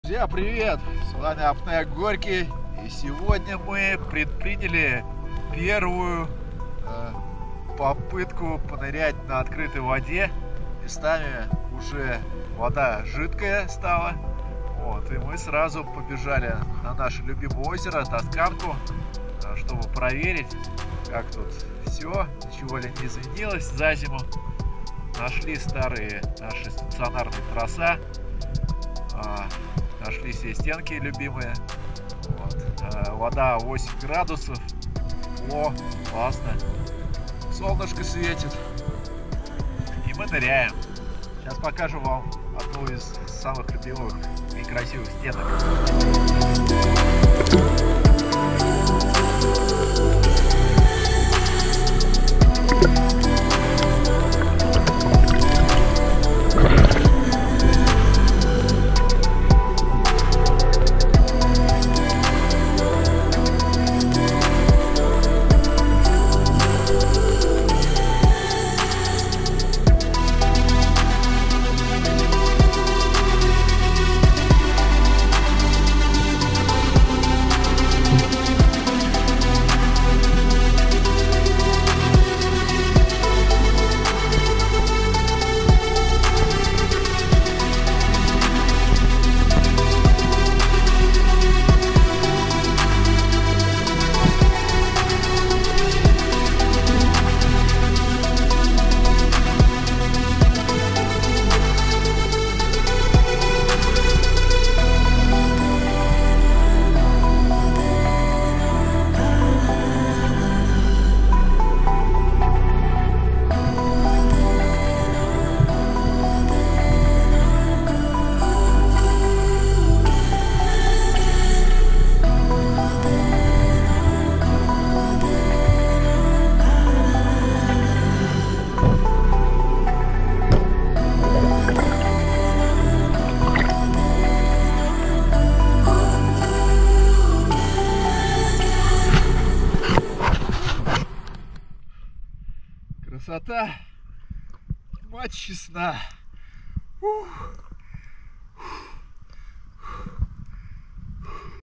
Нырнули на дно озеро и не поверили глазам!
Что скрывают воды озера Тосканка в Нижегородской области?
Посередине озера стоит монастырь 16 века.
А под водой необыкновенные пещеры!